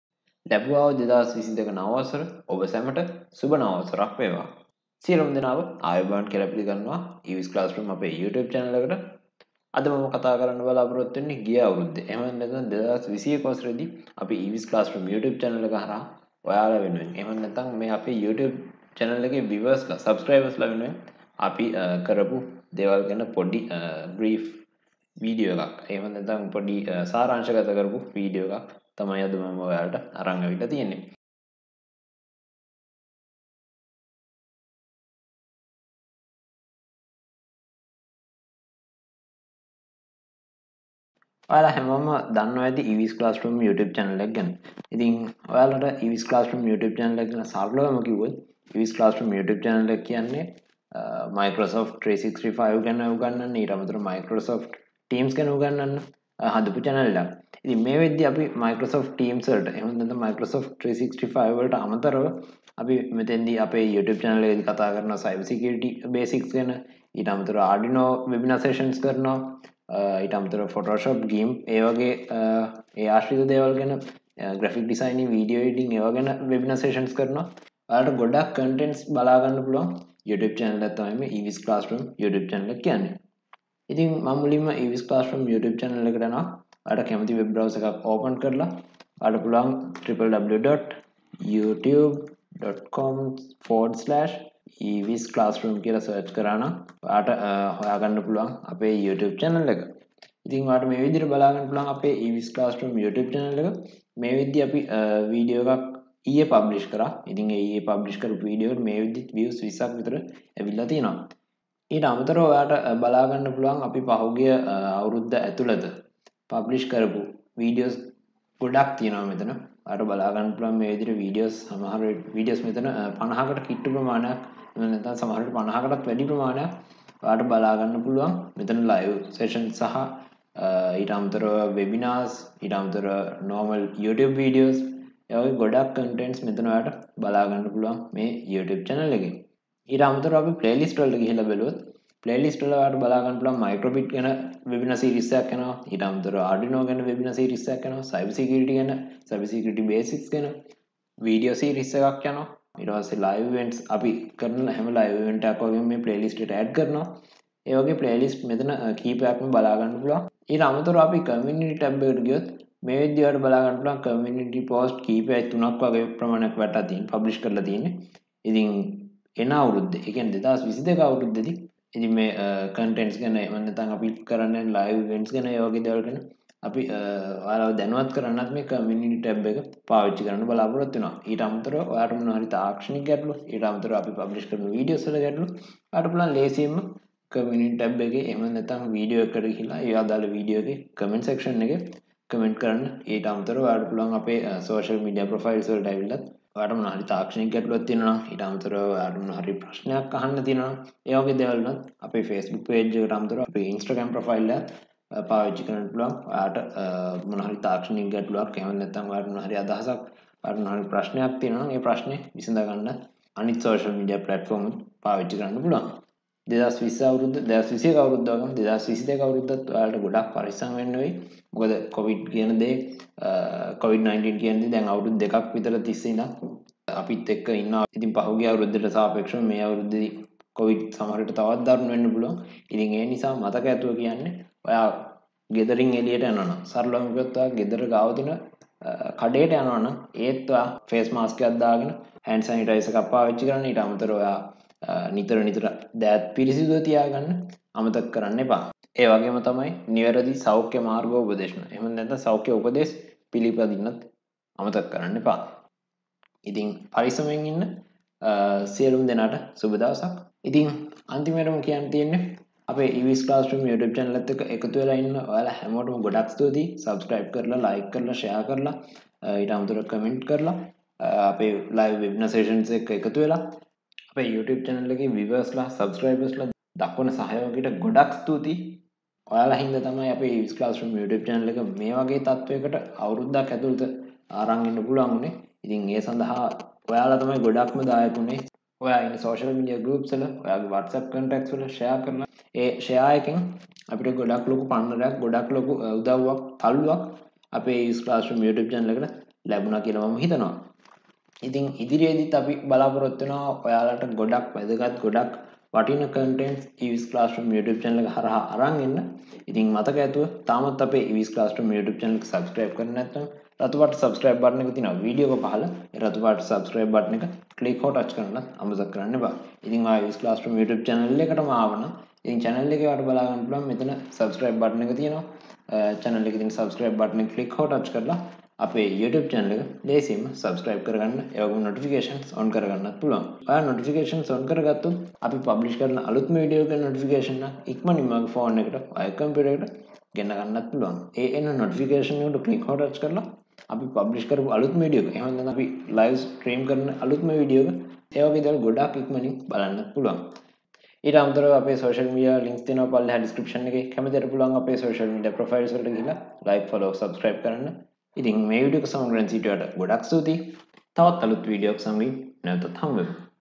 Happy New Year 2022 | EWIS Classroom YouTube journey in 2021 |Sinhala
Hello there, And welcome to our EWIS Classroom YouTube channel.
In this video, I am going to talk about the EWIS Classroom YouTube journey in 2021

Timestamps:
Happy New Year 2022 00:00
Intro 00:05
What is the EWIS Classroom YouTube channel is? 00:40
How to find our EWIS Classroom YouTube channel using web browser 01:23
About EWIS Classroom YouTube Channel 01:40
About Covid-19 Pandemic situation 03:36
Thank you very much for your valuable support 04:20
Ending 05:09

If you have any problems, let us know.
We will help you to solve that problem.

And you will not miss any future videos.
Stay safe and happy learning.